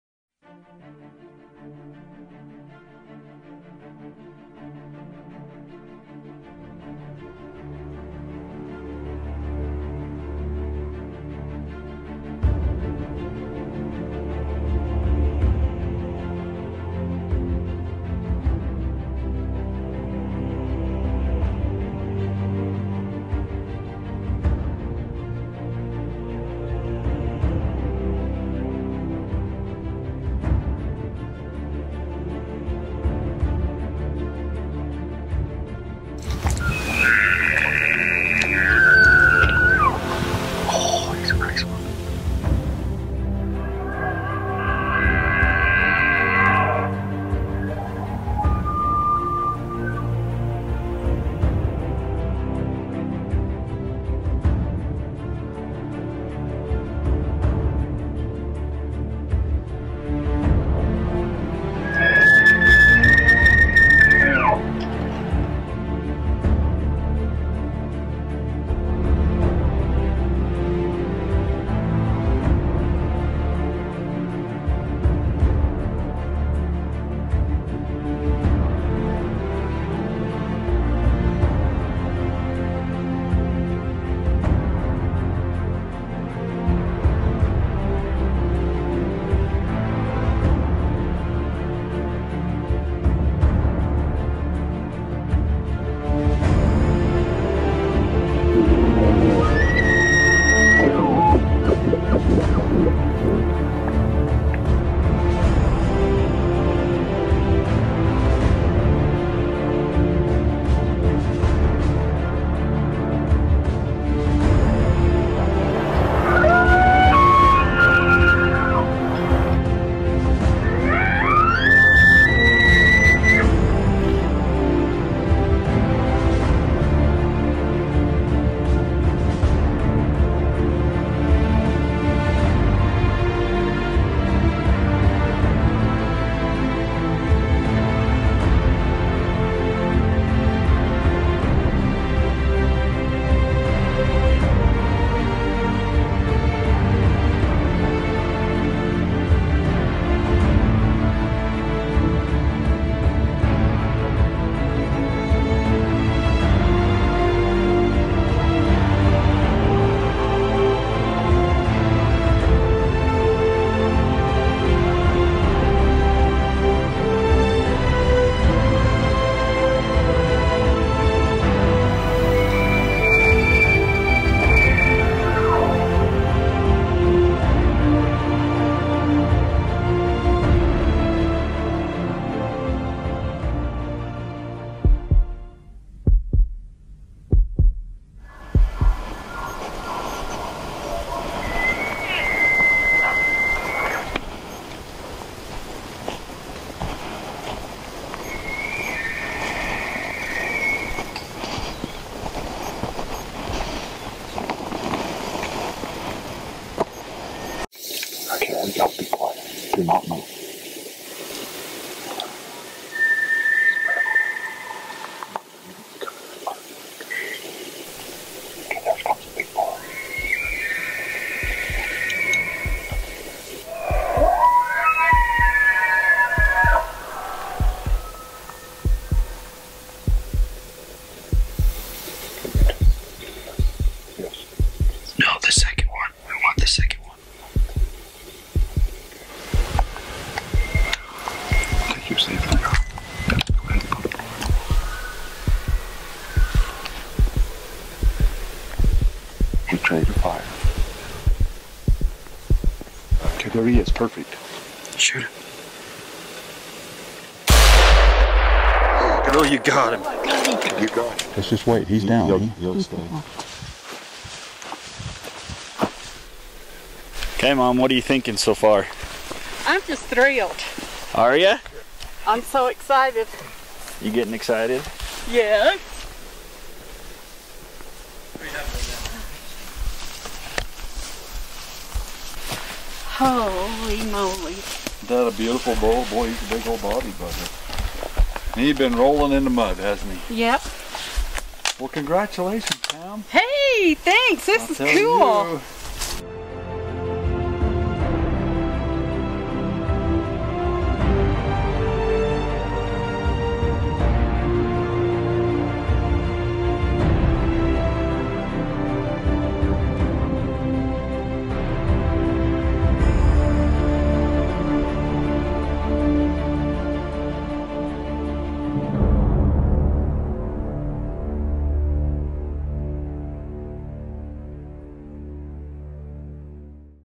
Southern Belle Bull Elk Hunt HD
In Fall  of 2014 Pam drew her first ever Bull elk tag. Her husband put her in for an hunting unit near Strawberry Reservoir for many years unbeknownst to her. One day spencer excitedly informed her of her winning a tag. Pam surprised, was  elated at the idea to see, hear and harvest an elk for the first time. Gary was her guide and a good one at that, over a 2 day period Gary guided her into some really nice bulls. She thanks him for the time and effort it took to achieve one of her dreams of not only hearing a elk bugle but also harvest a trophy. Pam was accompanied by her husband and their  son Trent who filmed, edited, and shared this video.